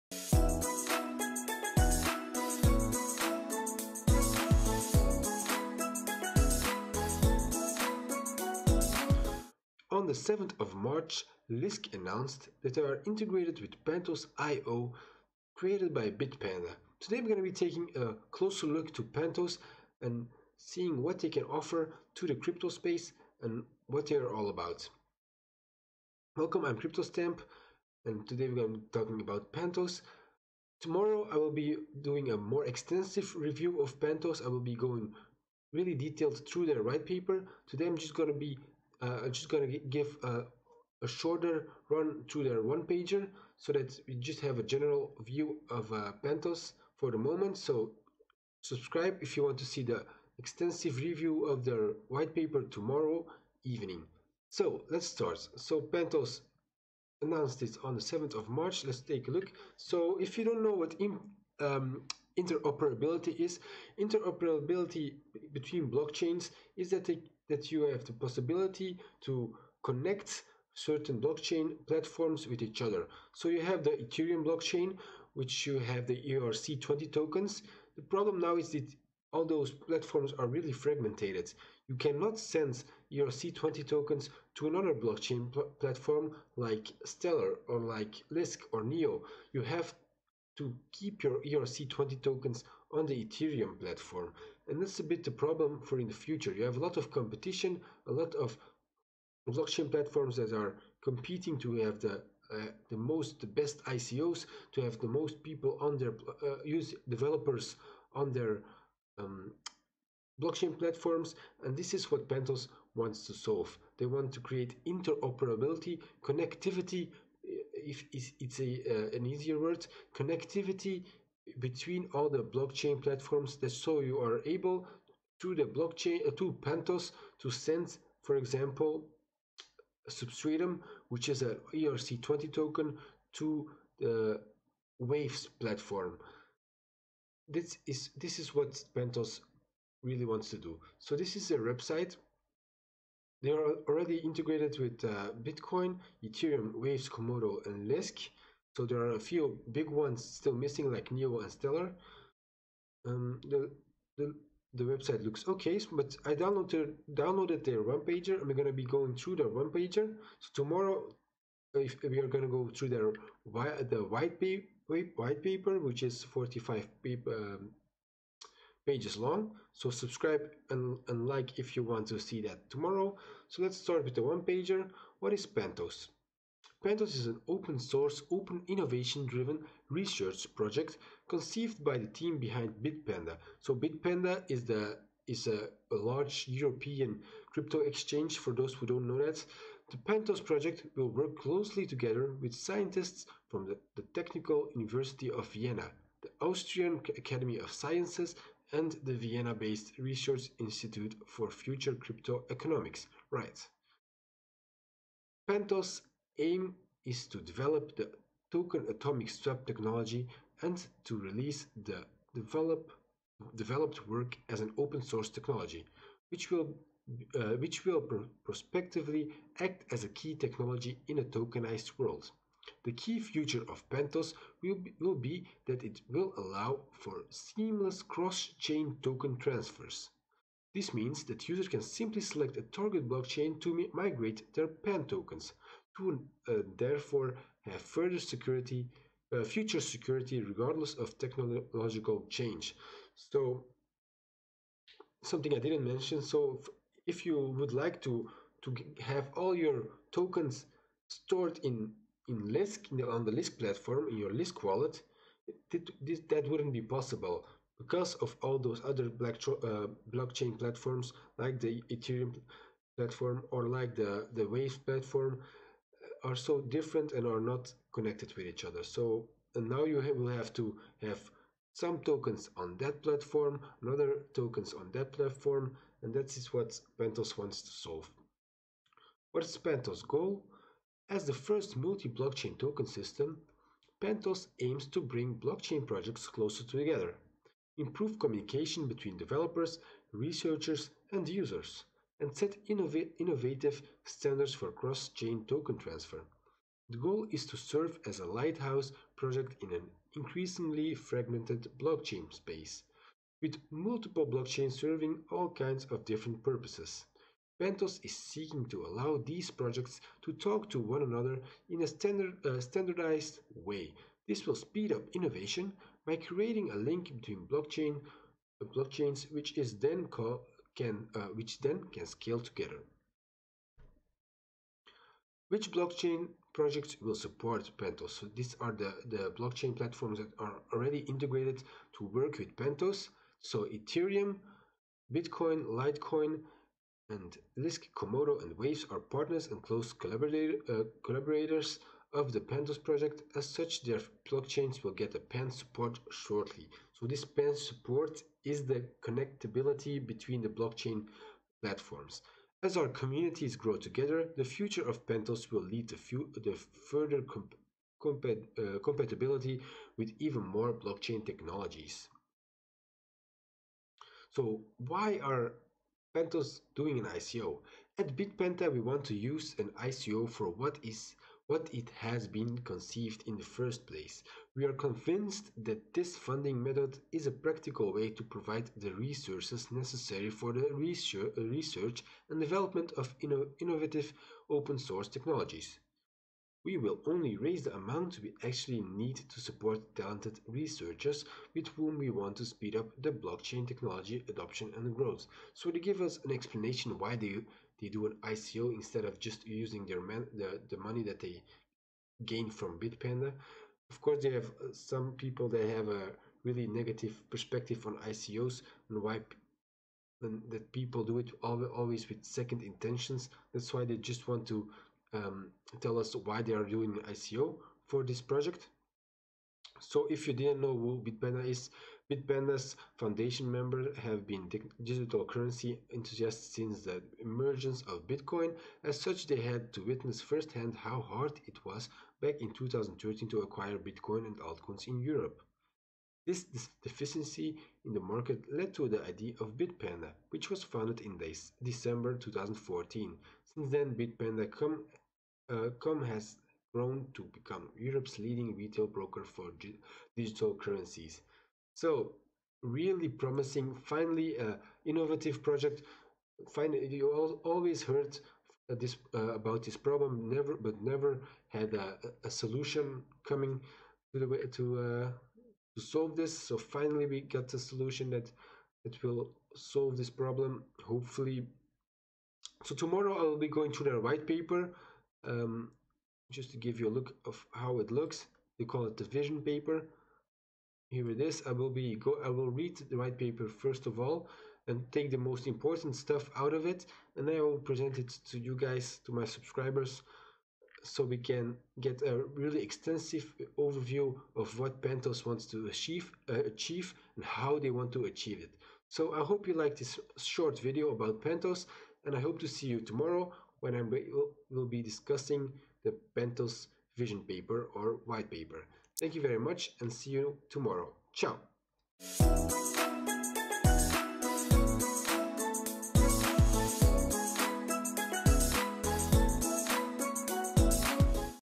Pantos - Connecting Blockchain Platforms - Lisk, Ethereum, Bitcoin, Litecoin, Waves, Komodo
Pantos want to create interoperability between blockchain platforms.
Here we take a look at the project and the things around it.